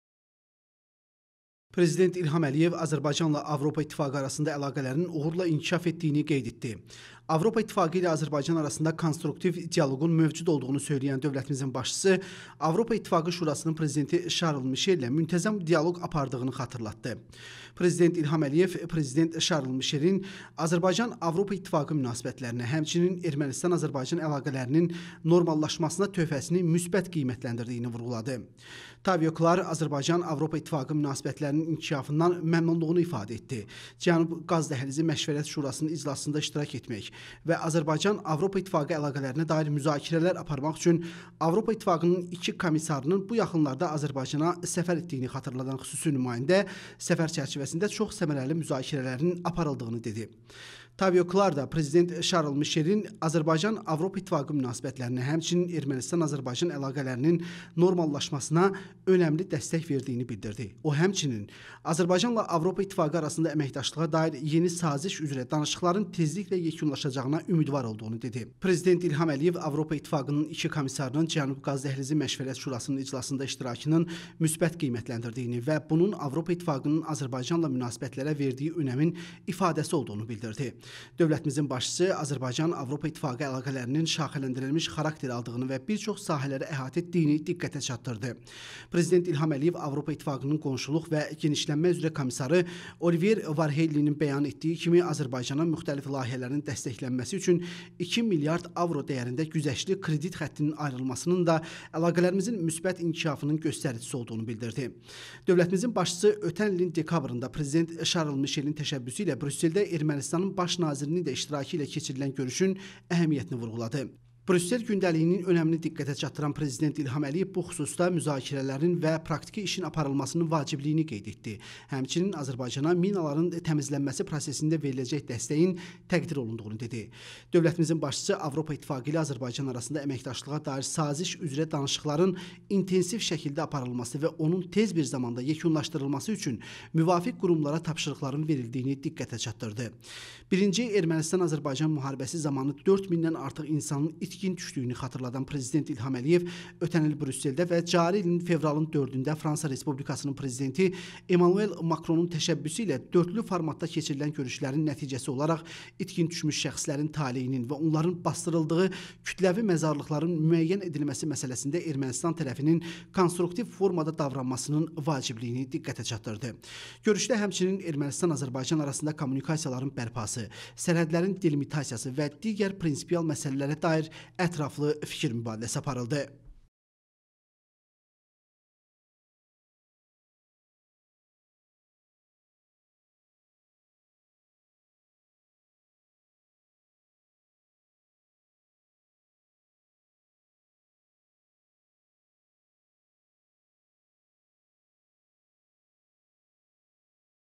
Prezident İlham Əliyev Avropa İttifaqının Cənubi Qafqaz üzrə xüsusi nümayəndəsini qəbul edib
Prezident İlham Əliyev bu gün Avropa İttifaqının Cənubi Qafqaz üzrə xüsusi nümayəndəsi Toivo Klaarın başçılıq etdiyi nümayəndə heyətini qəbul edib.